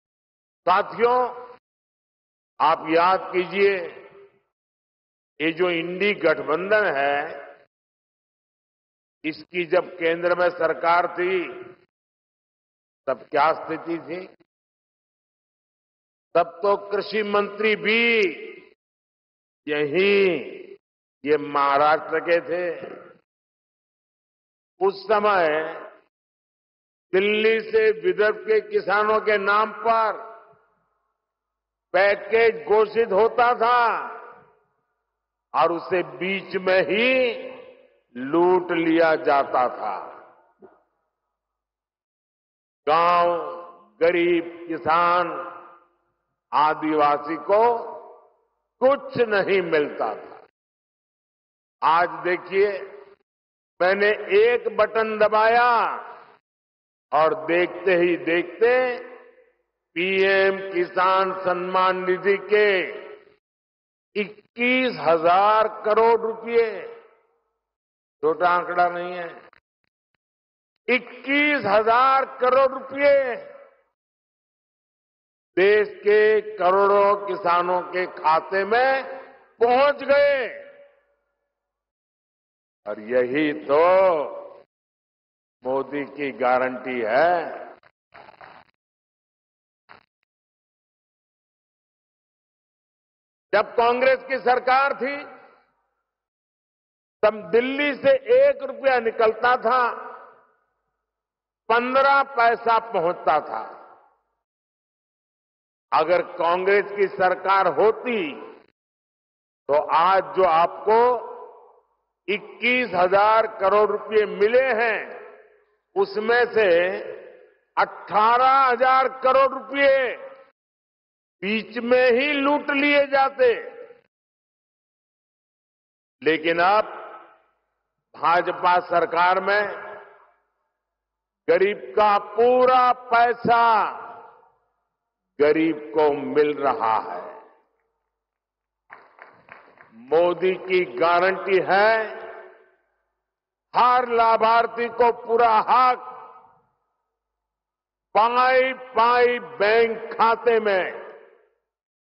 भाजपा सरकार में कोई भी किसानों का हक नहीं मार सकता - मा. प्रधानमंत्री नरेंद्र मोदी जी
( मा. मोदी जी द्वारा योजनाओं का उदघाटन | यवतमाल | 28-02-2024)